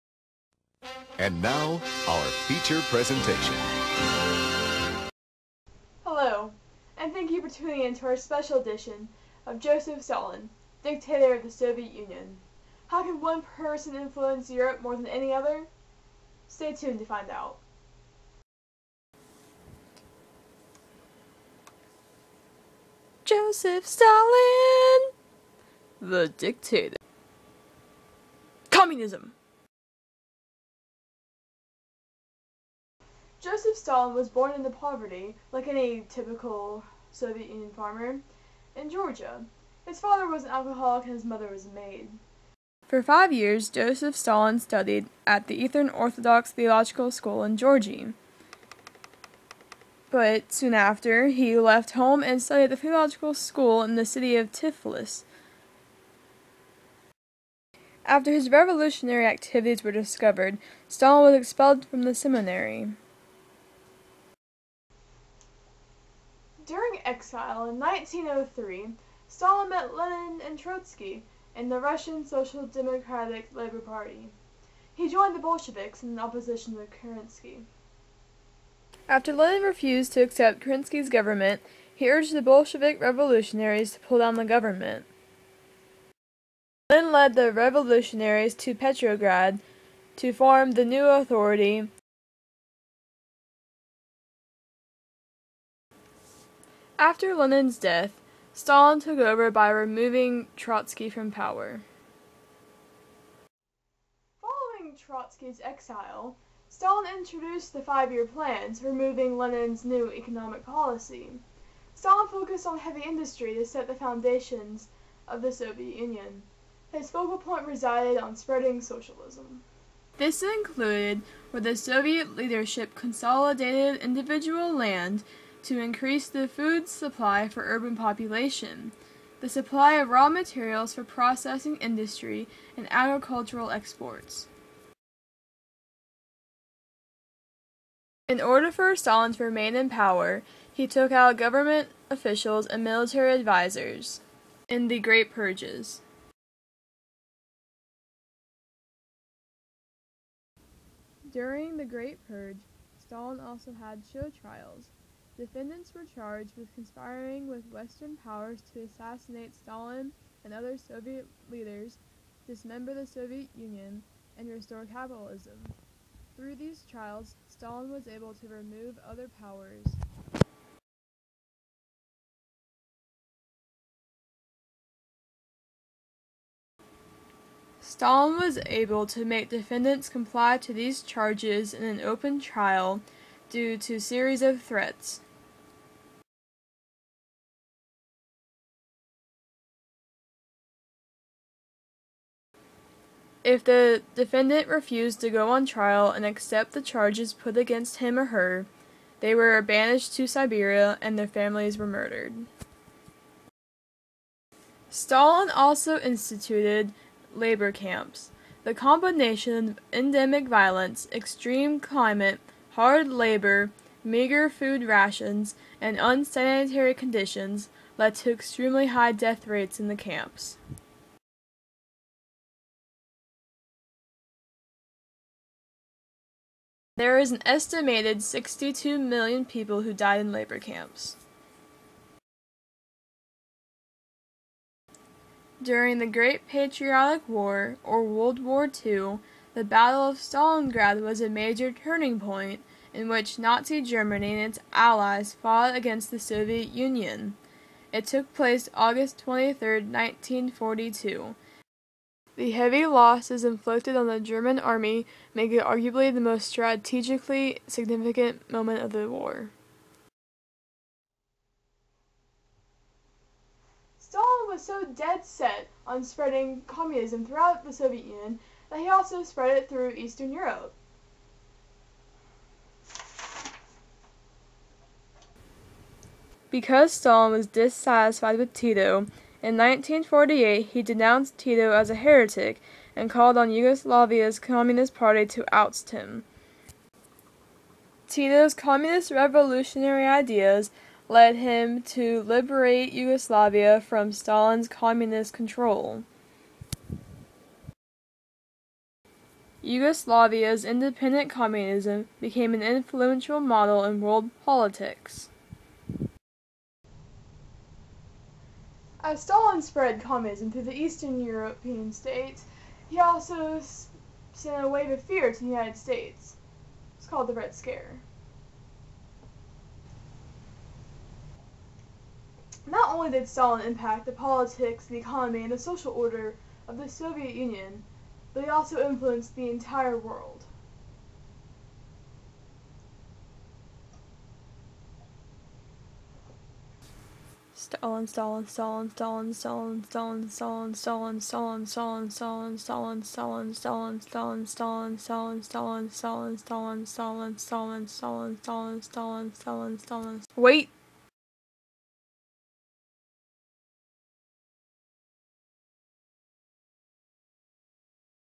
The Stalin Project
Project for AP European History
No one was harmed in the making of this film

Background information on Joseph Stalin's life, important actions made during his life, and the influential results after his life.

Green screen credit to Hank Hester